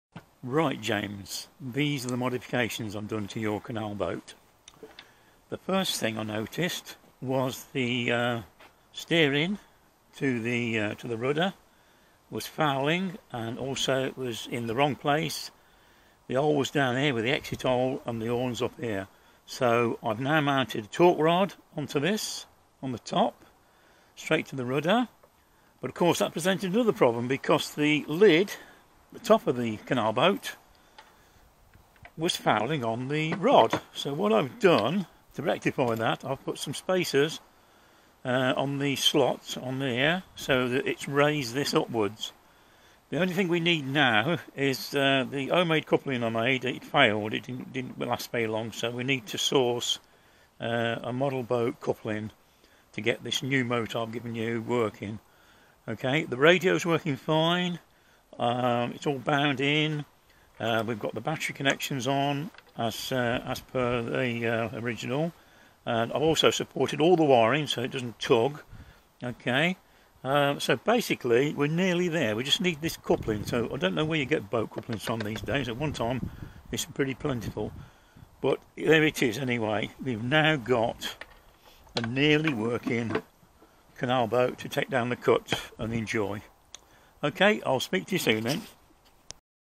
JAMES CANAL BOAT UPGRADES & CONVERSION
A model canal boat that was built some years ago,young James was having problems so I came to the rescue to upgrade and modify the boat to make it seaworthy again,the only thing that needs fixing now for the new motor is installation of a new universal joint motor coupling,as there are no model shops anywhere near me now I am not sure where we can get this item from ?